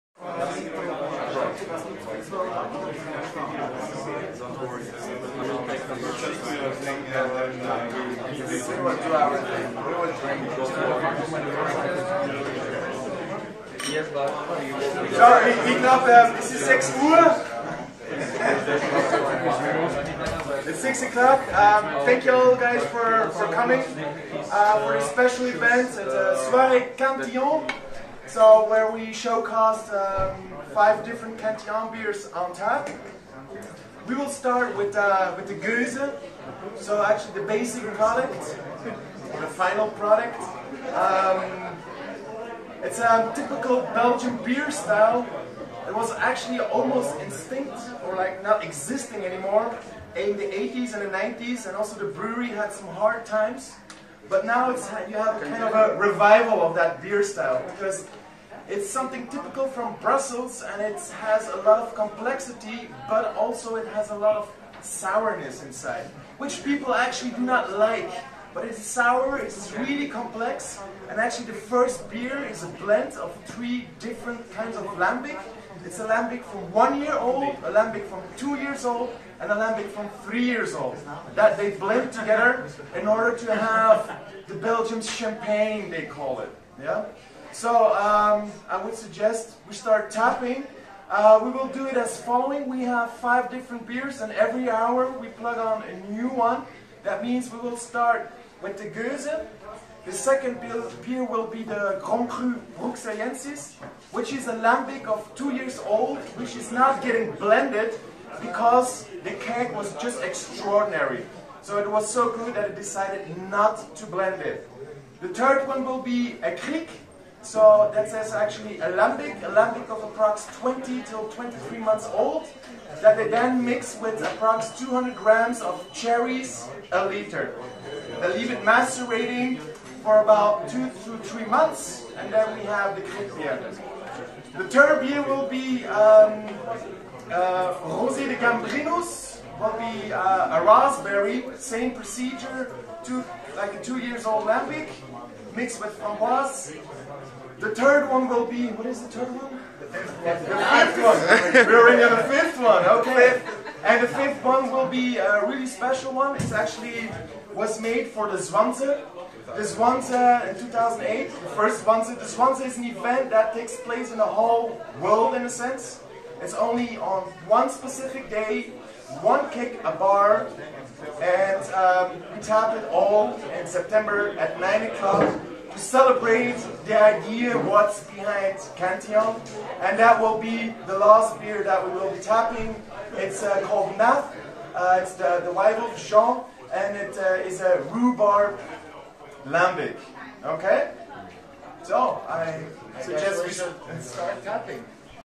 Soirée Cantillon.AmmutsØn. Vienna, May 8, 2018
A whole evening dedicated to spontaneous fermentation. Official release of the first keg (Gueuze) at AmmutsØn in Vienna.